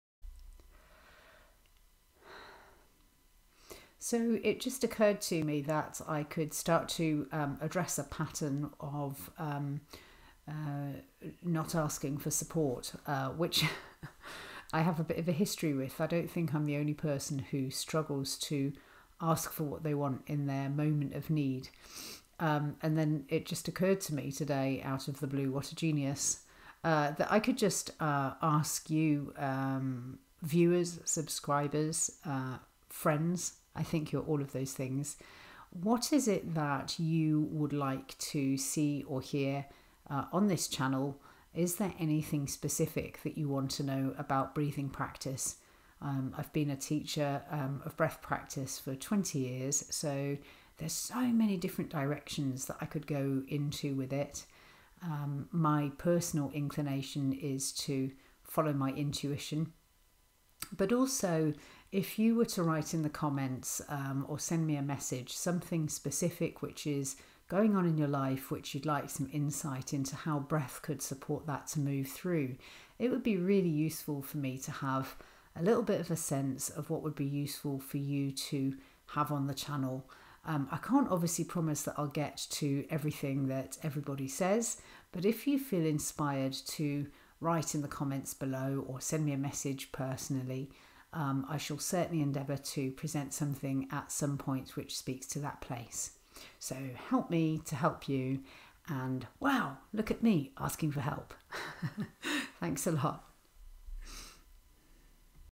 Support Me, to Support You
This is an invitation to post questions you may have about breathing practice into the comments section. Perhaps you have circumstances or re-occurring patterns in your life that you would like to resolve, and you are not sure whether conscious breathing could be a tool that could support you - why not ask! Thank you for supporting me to support you!